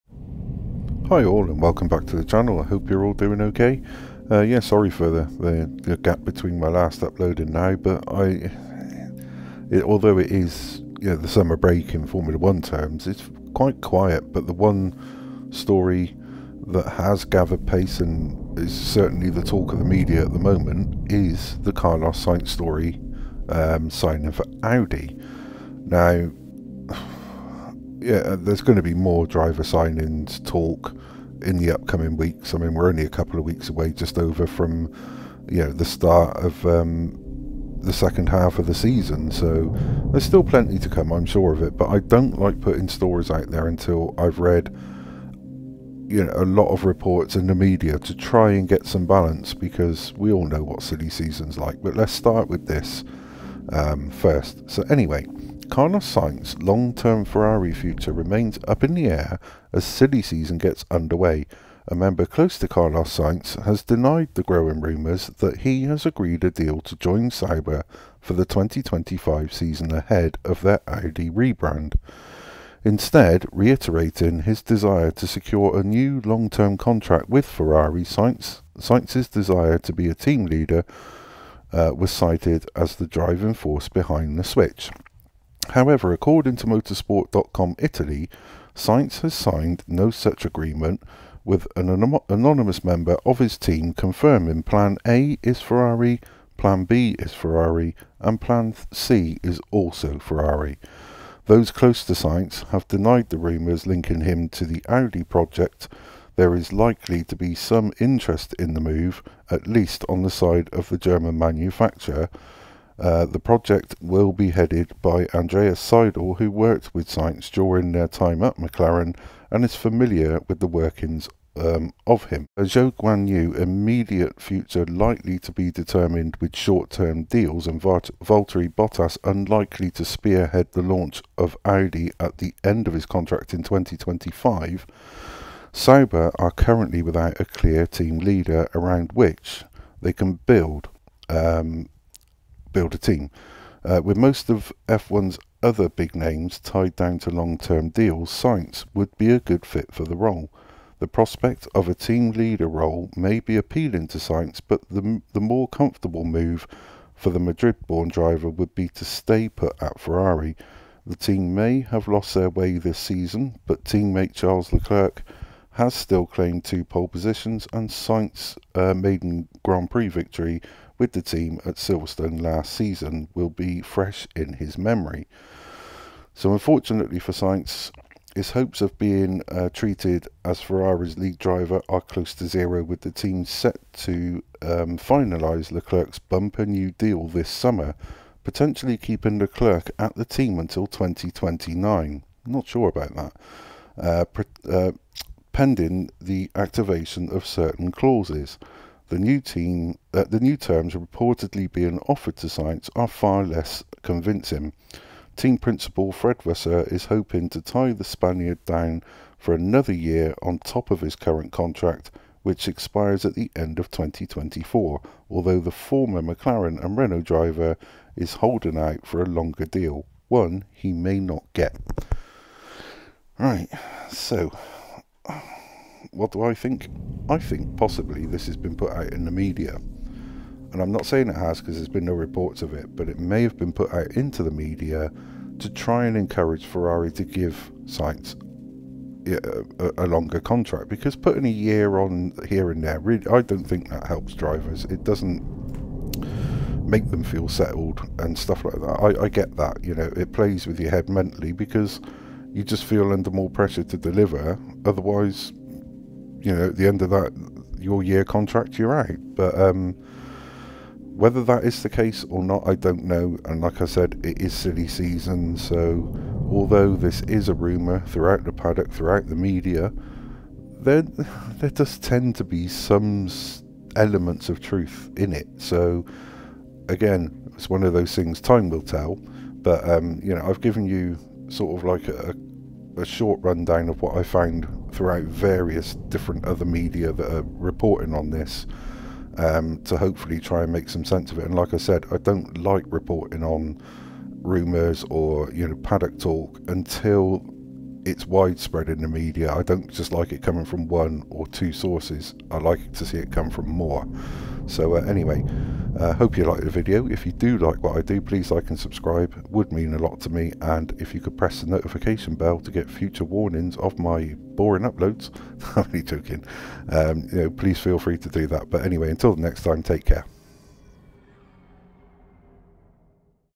Is Sainz’s move to Audi a “done deal” let’s discuss!
Hi and welcome back. I wanted to cover this after spending as much time as possible reading various different reports on the Sainz to Audi rumour, so here’s what I’ve come up with.